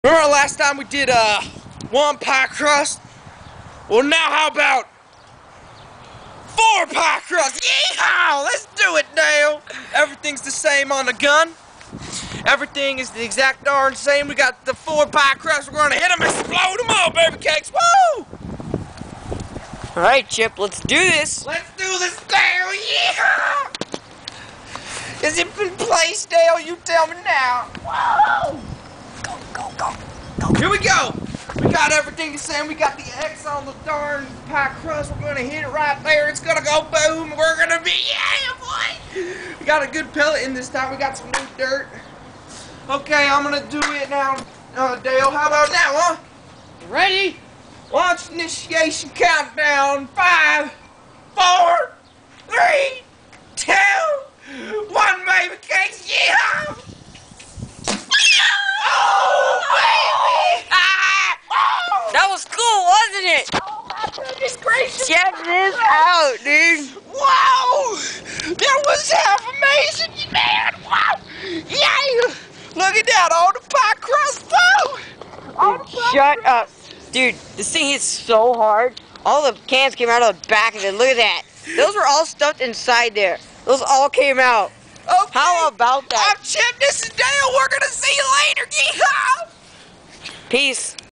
Water Cannon: Episode 5
Subject E: three pie crusts.
Starring (you know who): Chip and Dale Morris (with Texas accents)